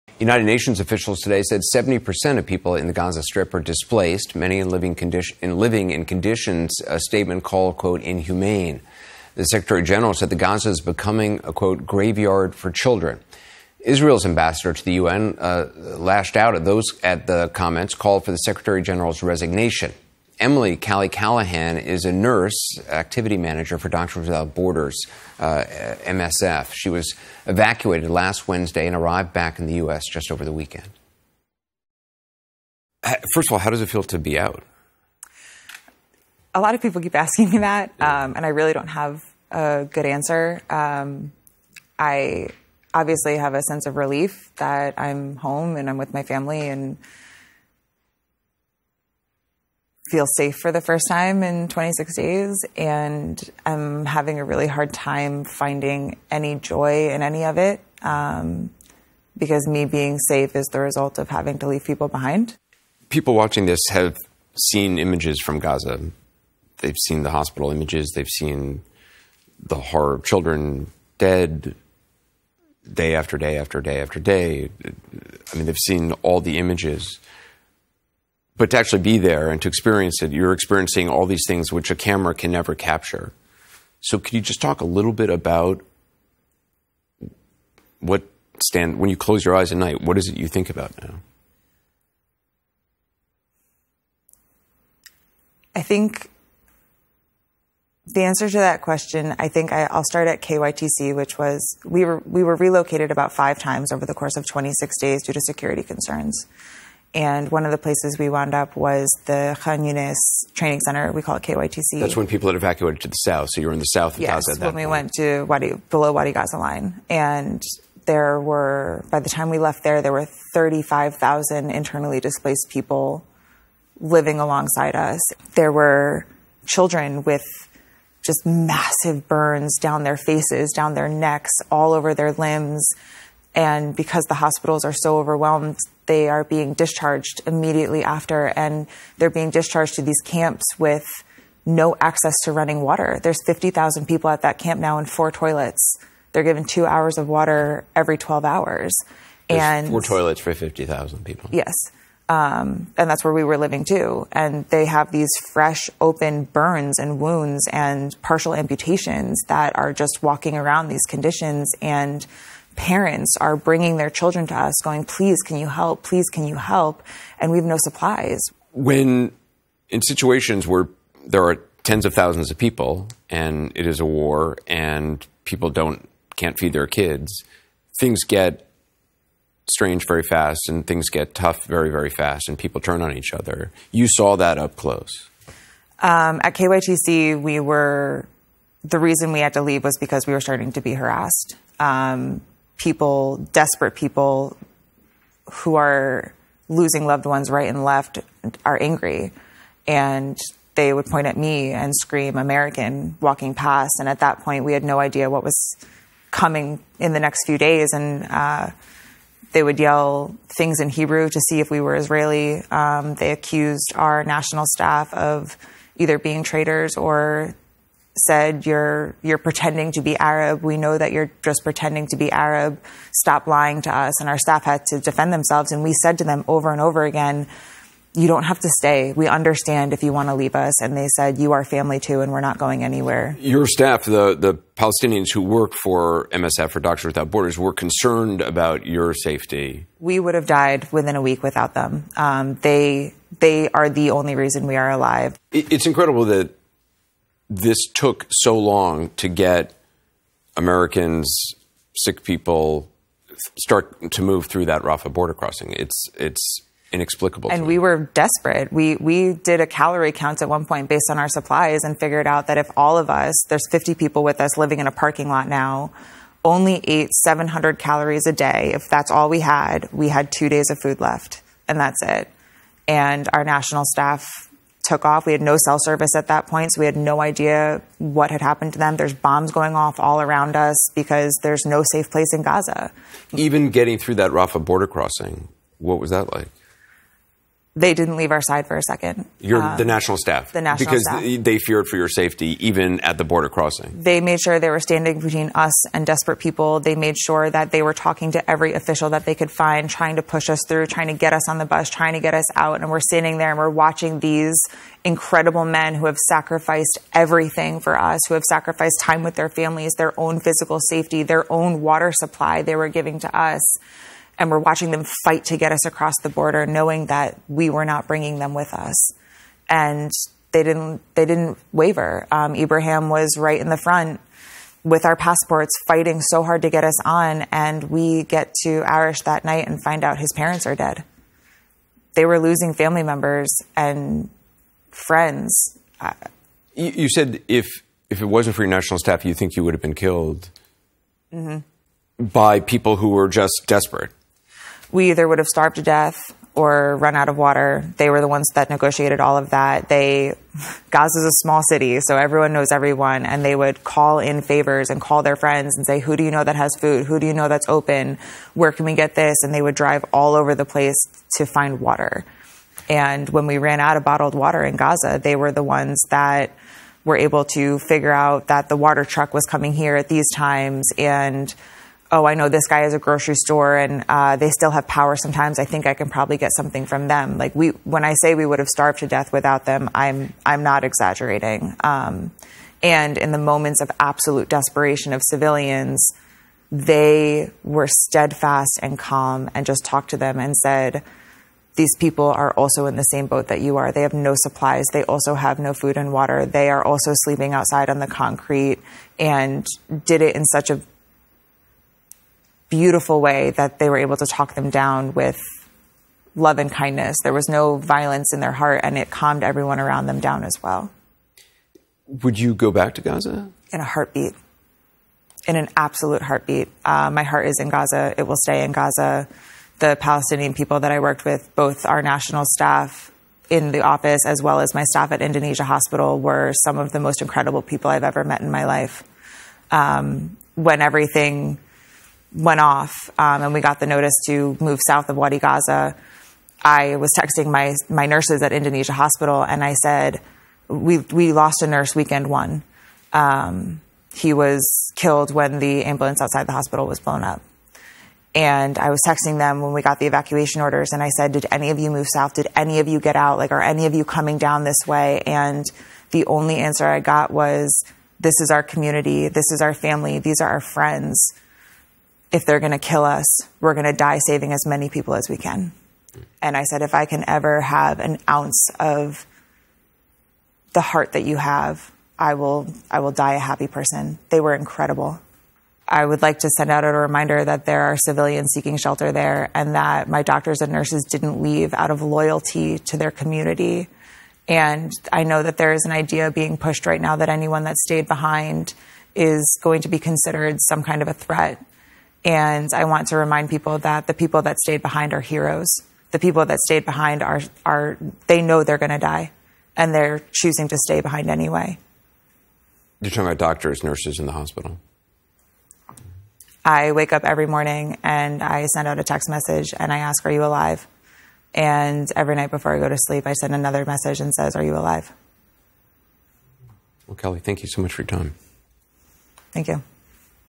ممرضة أمريكية تم اجلاؤها من غزة: لو امتلكت ذرة مما بقلوب من كنت معهم سأموت سعيدة.. هل تعود إلى هناك؟
وصفت مديرة نشاط التمريض في منظمة أطباء بلا حدود، إميلي كالي كالاهان، التي تم إجلاؤها من غزة الأربعاء الماضي، وعادت إلى الولايات المتحدة خلال نهاية الأسبوع، الأوضاع التي شهدتها في القطاع والصمود والهدوء الذي اتسمت به الطواقم الطبية في القطاع رغم معرفتهم بأنهم قد يتعرضون للموت، حسبما قالت لـCNN.

وقالت كالاهان إنهم كانوا "إما سنموت جوعًا أو بنفاد الماء. وهم الذين تفاوضوا على كل ذلك، إنهم - غزة مدينة صغيرة. لذلك يعرف الجميع بعضهم، وكانوا يتفضلون ويتصلون بأصدقائهم ويقولون: من تعرف أنه لديه طعام؟ من تعرف أن متجره مفتوح؟ أين يمكننا الحصول على هذا أو ذلك؟ كانوا يتجولون في كل مكان بحثًا عن الماء. عندما نفدت المياه المعبأة لدينا في غزة، كانوا هم الذين تمكنوا من معرفة أن شاحنة المياه كانت قادمة في الوقت المعين".

وتابعت بالقول: "(بعضهم كان يقول) أعلم أن هذا الرجل يعمل في متجر بقالة، وما زالوا يملكون الطاقة في بعض الأحيان، أعتقد أنه ربما يمكنني الحصول على شيء منهم. عندما أقول إننا كنا سنموت جوعًا بدونهم فأنا لا أبالغ. وفي لحظات اليأس المطلق للمدنيين، كانوا صامدين وهادئين، يتحدثون معهم ويقولون لهم إن هؤلاء الأشخاص أيضًا يعيشون نفس الأوضاع، ليس لديهم الإمدادات وليس لديهم طعام وماء، إنهم ينامون أيضًا في الخارج على الخرسانة، وقد فعلوا ذلك بطريقة جميلة بحيث تمكنوا من تهدئتهم بالحب واللطف. لم يكن هناك عنف في قلوبهم، وكان ذلك يهدئ كل من حولهم أيضًا".

وعند سؤالها عما إن كانت ستعود إلى غزة، ردت كالاهان بالقول: "على الفور، على الفور بالتأكيد. قلبي في غزة وسيبقى في غزة".

وأشارت إلى أن "الشعب الفلسطيني الذي عملت معه - كل من موظفينا الوطنيين في المكتب وكذلك الموظفين العاملين لدى المستشفى الإندونيسي من أكثر الأشخاص الرائعين الذين قابلتهم في حياتي. عندما انطفأ كل شيء وحصلنا على إشعار بالتحرك جنوب وادي غزة، كنت أرسل رسالة نصية إلى الممرضات في المستشفى الإندونيسي وقلت - كنا قد فقدنا ممرضًا في عطلة نهاية الأسبوع. لقد قُتل عندما انفجرت سيارة الإسعاف خارج المستشفى. كنت أرسل لهم رسالة نصية عندما حصلنا على أوامر الإخلاء فقلت: هل انتقل أحد منكم إلى الجنوب؟ هل خرج أحد منكم؟هل سيأتي أحد منكم إلى هذا الجانب؟ والجواب الوحيد الذي حصلت عليه هو: هذا هو مجتمعنا، وهذه هي عائلتنا، وهؤلاء هم أصدقاؤنا. إذا كانوا سيقتلوننا، فسنموت لإنقاذ أكبر عدد ممكن من الناس".

واستطردت بالقول: "وقلت، إذا كان بإمكاني الحصول على ذرة مما في قلوبكم، فسوف أموت سعيدة. لقد كانوا لا يصدقون".

وأنهت كالاهان تعليقها بالقول: "أود أن أرسل تذكيرًا بأن هناك مدنيين يبحثون عن مأوى هناك وأن الأطباء والممرضات لم يغادروا المكان بسبب ولائهم لمجتمعهم. وأنا أعلم أن هناك فكرة يتم الترويج لها الآن مفادها أن أي شخص يتخلف عن الركب سيُعتبر بمثابة تهديد من نوع ما. وأريد أن أذكّر الناس بأن الأشخاص الذين بقوا في الخلف هم أبطال. الأشخاص الذين بقوا في الخلف يعرفون أنهم سيموتون ويختارون البقاء على أي حال".